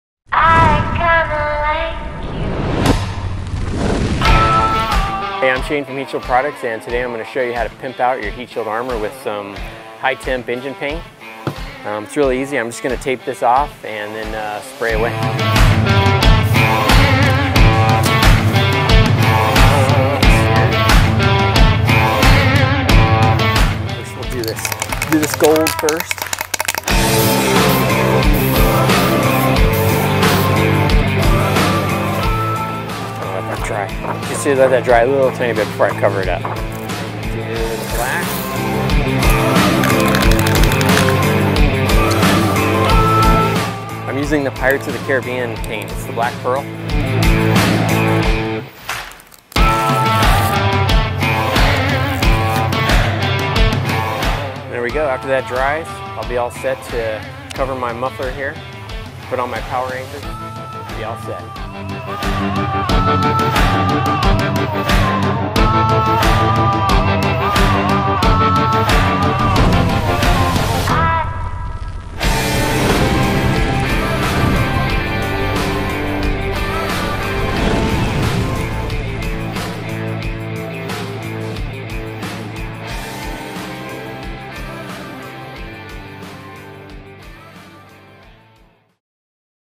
Customizing Heatshield Armor
The Heatshield Products Armor Series can be fully custom. You can go as mild or as wild as you want. We show a quick tip here where we paint with some high-temperature engine paint.  Using the Header Armor instead of an exhaust wrap can stop more heat and keep you warranty compliant for most header manufactures. Using this manifold heat shield you can stop up to 70% of exhaust related heat under the hood.

Go ahead and grab a hold of your inner Michelangelo, now you can keep cool while looking cool.

Products used in video:
Exhaust Heat Shield – Muffler Armor
SKU: 177101

Heatshield Products, Inc.